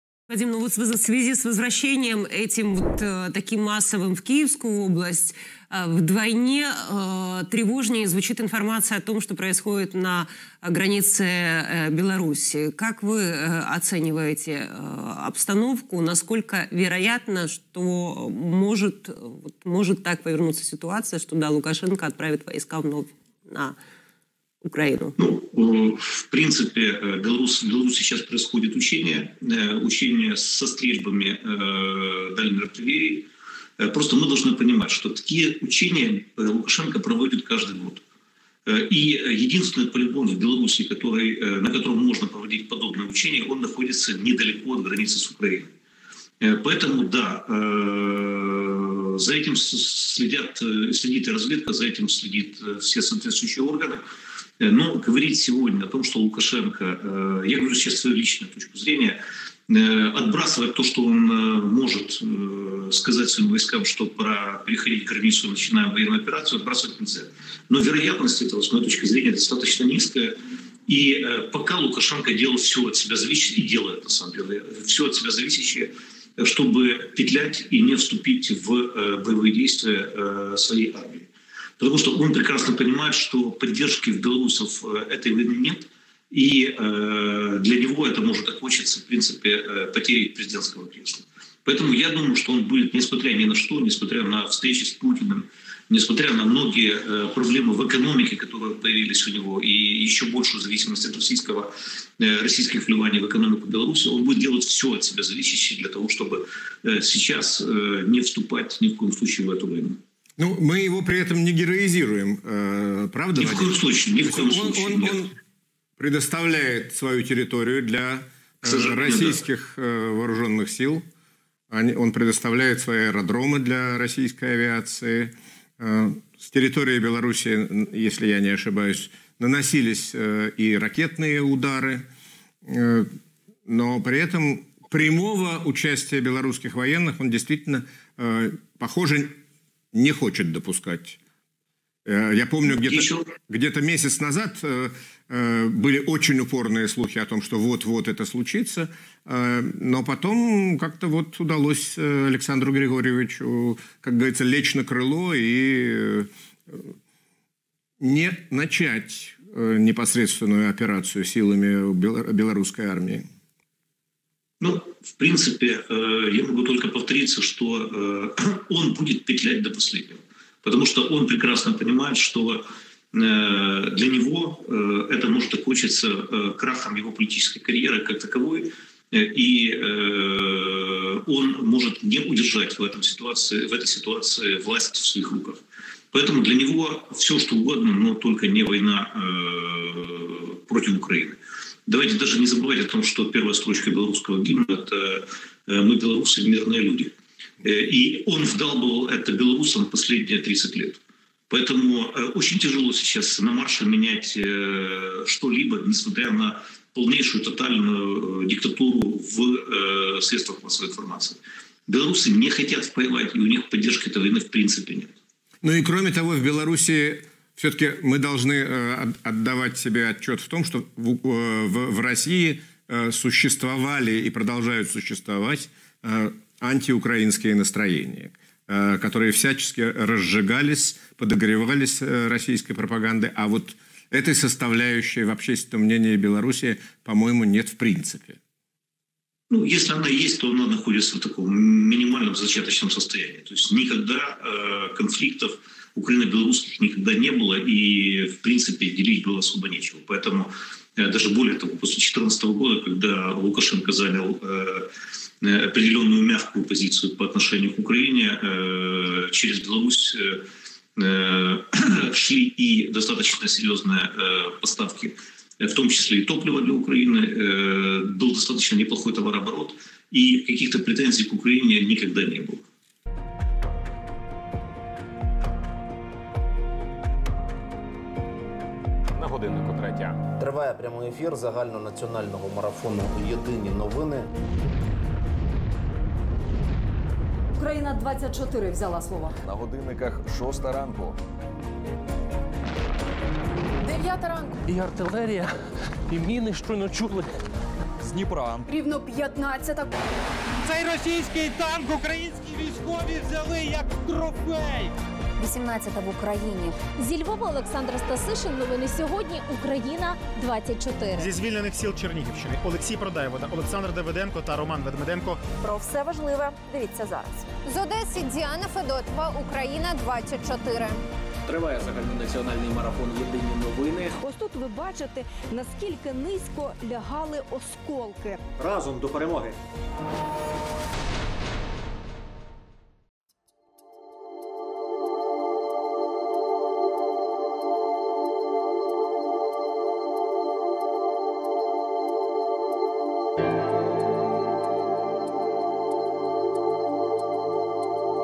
⚡️Лукашенко дуже боїться! Денисенко пояснив, чому Білорусь не нападе / розвідка, зрада / Україна 24
Радник міністра внутрішніх справ України Вадим Денисенко розповів, що у Білорусі зараз проводяться навчання, за цим ретельно стежить розвідка.